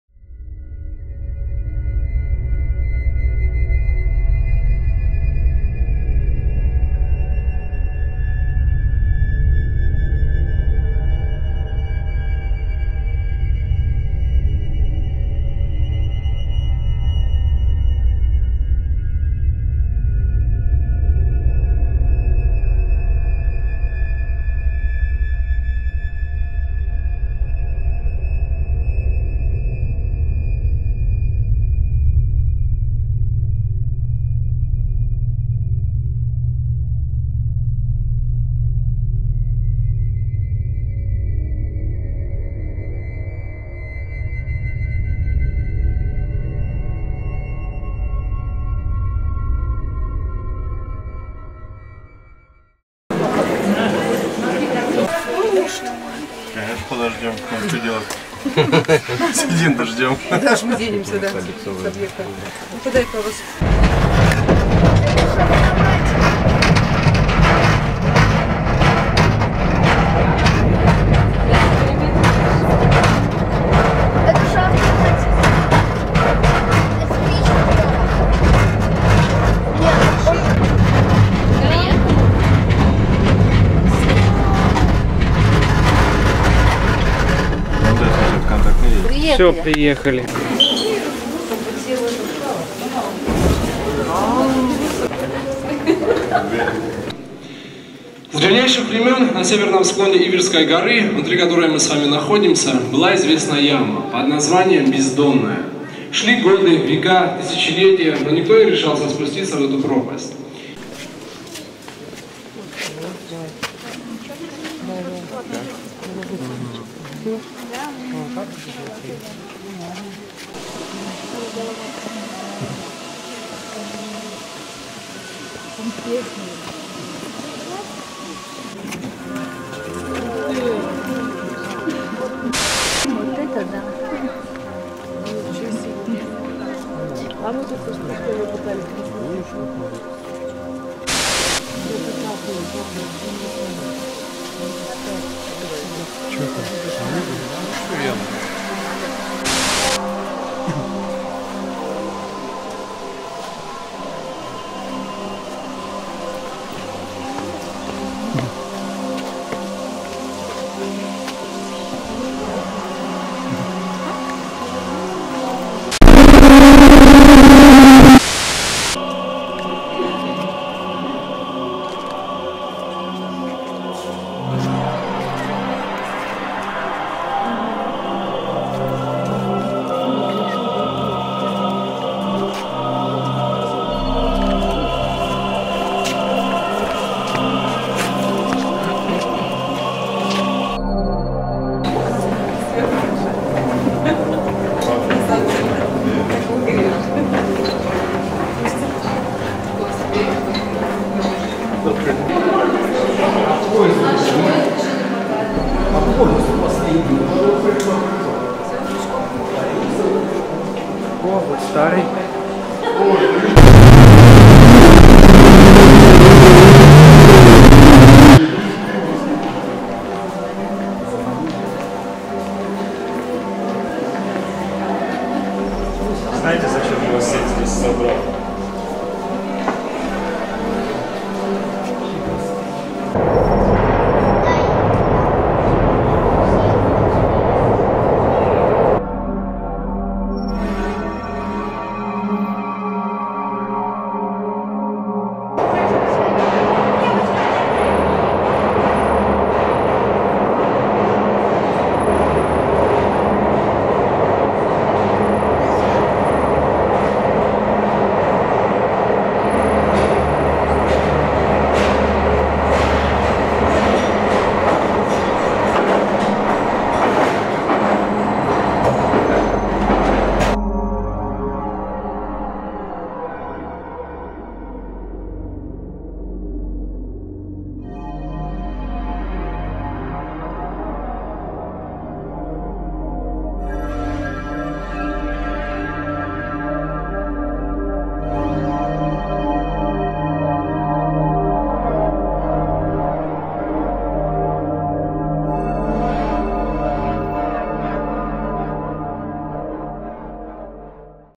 Пещера / Cave (2015)
Сюжет: Этот фильм состоит из кадров, снятых летом 2015 года в Новоафонской пещере (Абхазия), когда люди увидели нечто паранормальное.

Всё сделал: Сергей А.
В ролях: Сергей А., какие-то люди

Жанр: Мокьюментари, Триллер, Ужасы